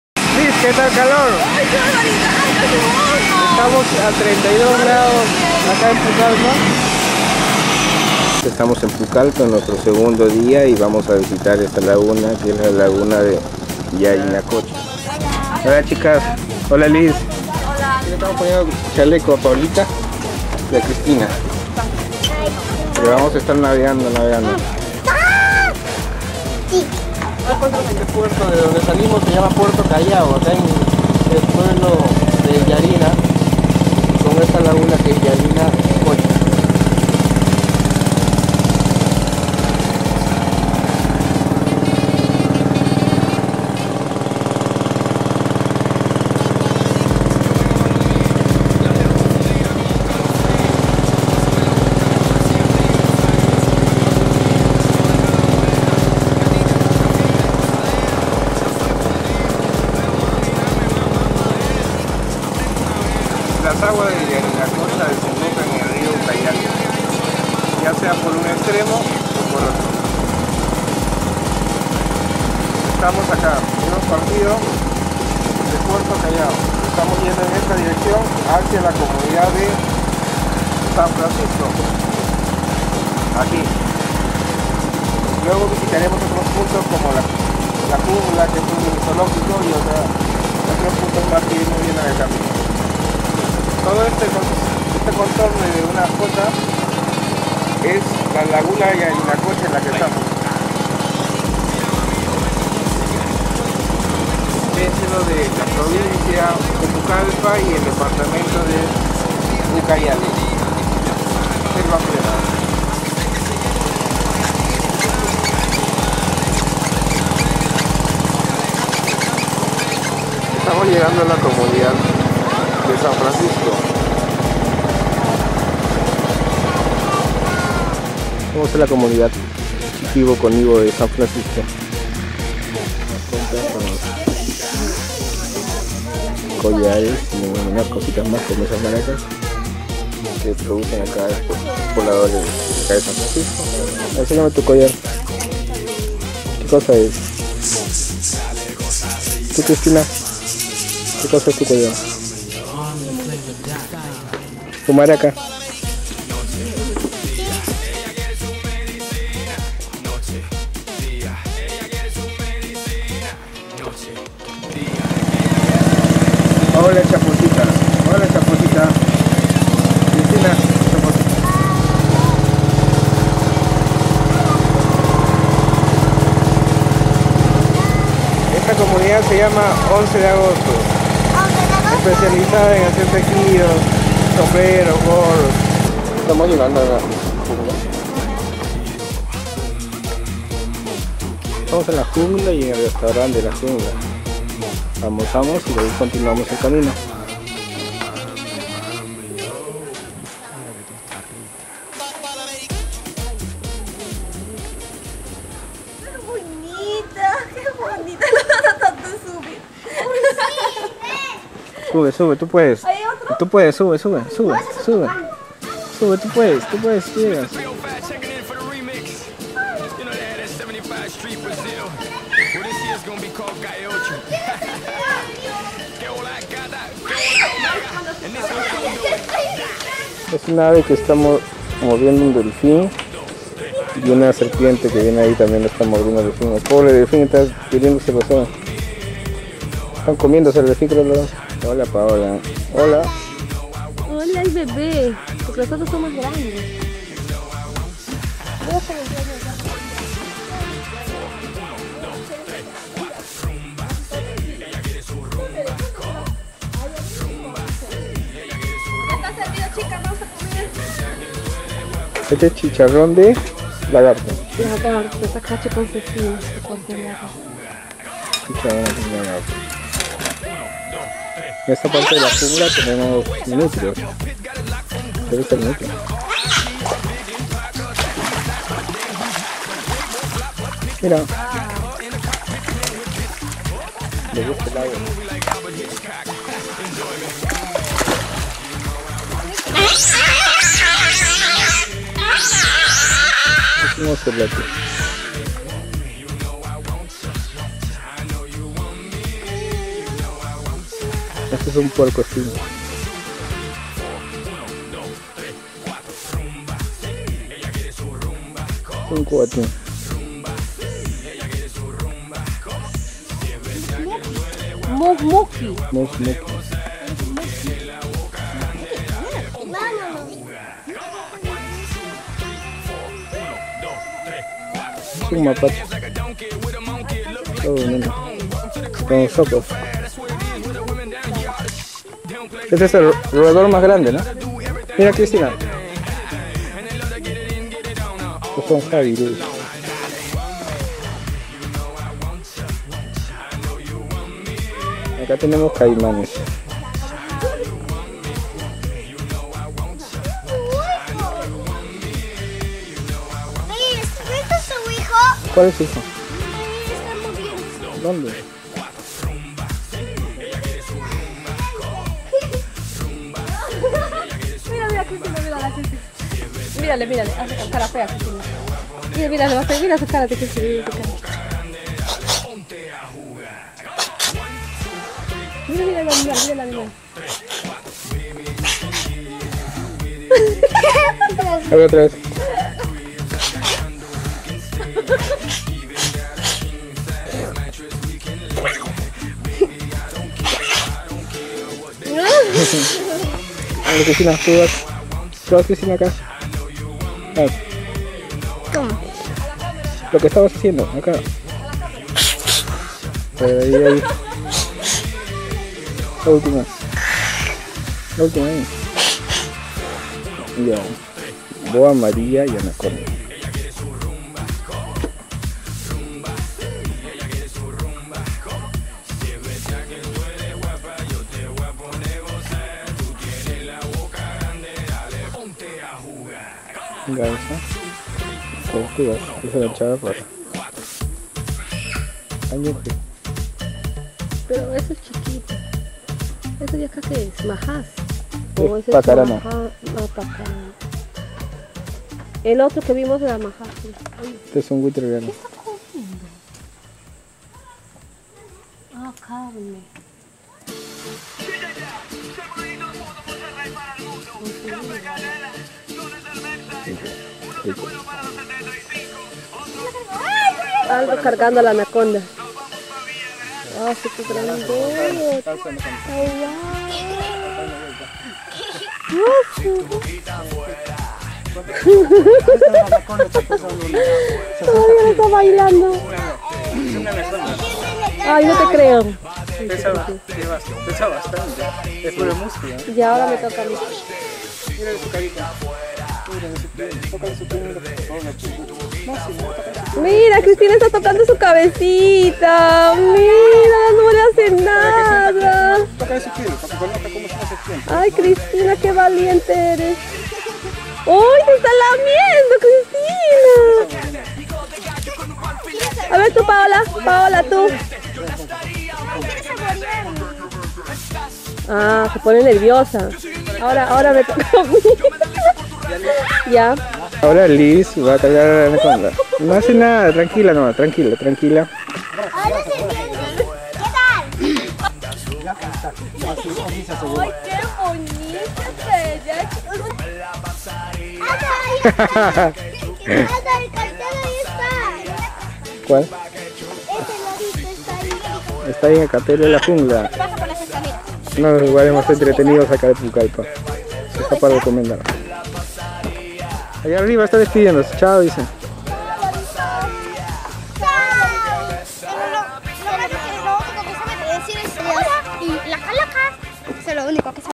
Pucallpa
navegando por la laguna de Yarinacocha encontramos la comunidad de San Francisco y el restaurante zoologico La Jungla.